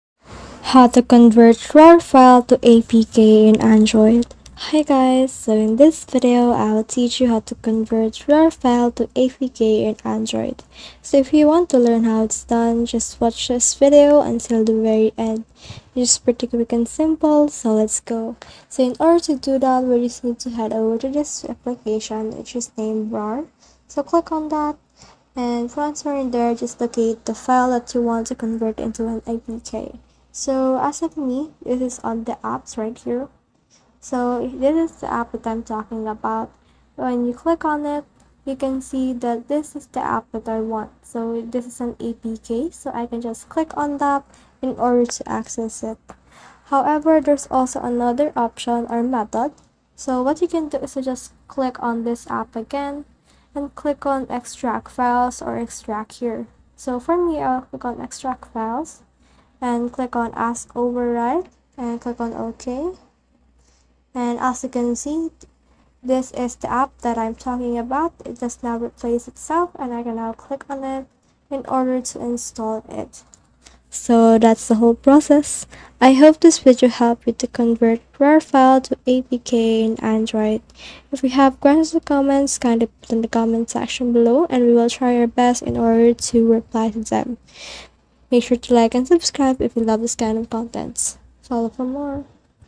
How To Convert RAR File To APK In Android
Are you looking for a way to convert RAR files to APK on your Android device? Look no further! In this comprehensive guide, we'll show you how to easily convert RAR files to APK format right on your Android smartphone or tablet.

Learn how to open and extract RAR files on Android, allowing you to access the contents of compressed files effortlessly. With our step-by-step instructions, you'll be able to unzip and extract files from RAR archives in no time.

Discover the best methods and apps to open RAR files on Android. We'll introduce you to reliable and user-friendly apps that will make the conversion process a breeze.

Unleash the power of your Android device and learn how to convert RAR files to APK. Whether you want to access an application or game stored in a RAR archive, our guide will provide you with the knowledge and tools to do so.

Follow our easy-to-understand instructions and learn how to extract RAR files on Android in a matter of minutes. Say goodbye to compatibility issues and enjoy the freedom of accessing APK files directly on your Android device.

Share this helpful video with your friends and fellow Android users.